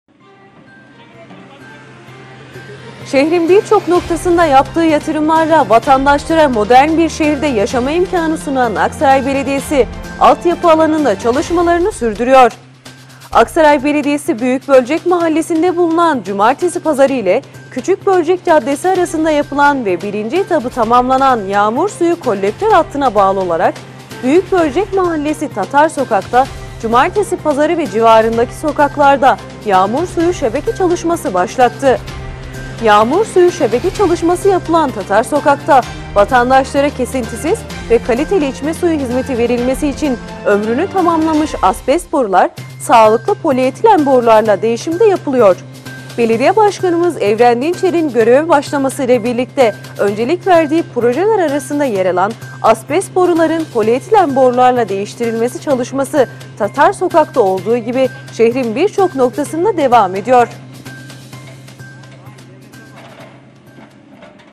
AKSARAY BELEDİYESİ ALT YAPI ALANINDA ÇALIŞMALARINI ARALIKSIZ SÜRDÜRÜYOR
BAŞKAN DİNÇER; ‘’ VATANDAŞLARIMIZ HER ŞEYİN EN İYİSİNE LAYIKTIR DÜŞÜNCESİYLE HAREKET EDİYORUZ’’

Şehrin birçok noktasında yaptığı yatırımlarla vatandaşlara modern bir şehirde yaşama imkânı sunan Aksaray Belediyesi, Alt yapı alanında çalışmalarını sürdürüyor.

Aksaray Belediyesi, Büyük Bölcek Mahallesinde bulunan Cumartesi Pazarı ile Küçük Bölcek Caddesi arasında yapılan ve birinci etabı tamamlanan Q1000 lik yağmur suyu kollektör hattına bağlı olarak; Büyük Bölcek Mahallesi Tatar Sokakta, Cumartesi Pazarı ve civarındaki sokaklarda yağmur suyu şebeke çalışması başlattı.

Öncelikli olarak ana cadde görevi gören Tatar Sokak üzerinde yağmur suyu şebekesine başlayan kanalizasyon ekipleri, burada Q600 lük koruge boruyla 576 metre, Q400 lük koruge boruyla 374 metre ve bu hatlara bağlanarak ara sokaklarda Q300 lük koruge boruyla 3725 metre yağmur suyu şebekesi yapacak. Şehrin alt yapı sorunlarına kalıcı çözümler üreten Aksaray Belediyesi toplamda belirlenen bu lokasyonlarda 4648 metre yağmur suyu şebekesi yapmayı planlıyor.

Yağmur suyu şebeke çalışması yapılan Tatar Sokak’ta vatandaşlara kesintisiz ve kaliteli içme suyu hizmeti verilmesi için ömrünü tamamlamış asbest borular sağlıklı polietilen borularla değişimi de yapılıyor. Belediye Başkanı Evren Dinçer’in göreve başlamasıyla birlikte öncelik verdiği projeler arasında yer alan astbest boruların politein borularla değiştirilmesi çalışması Tatar Sokak’ta olduğu gibi şehrin birçok noktasında devam ediyor.

Yapılan alt yapı çalışmaları ile değerlendirmelerde bulunan Belediye Başkanı Evren Dinçer yaptığı açıklamalarda şu ifadelere yer verdi; “Aksaray’ın sadece bugününü değil geleceği için de çalışıyoruz. Büyük Bölcek Mahallesi Tatar sokakta öncelikle yağmur suyu kanallarını döşüyoruz. Ardından ömrünü tamamlamış 40 yıllık içme suyu asbestli boruları sağlıklı polietilen borularla değiştiriyoruz. Hayata geçirdiğimiz ve vaat ettiğimiz her projede; vatandaşlarımız her şeyin en iyisine layıktır düşüncesiyle hareket ediyoruz.  Göreve başladığımız günden bugüne kadar geçen süreçte İçme suyu hatlarında yer alan asbestli boruları, polietilen boruları ile değiştirmeye başladık. Belirlediğimiz bir program dahilinde çalışmalarımızı sürdürüyoruz. Her çalışma dönemimizde kısım kısım yenileme yaparak şehrimizi asbestli borulardan kurtaracağız. Çünkü şehrimizin üst yapısı kadar alt yapısını da önemsiyoruz” dedi.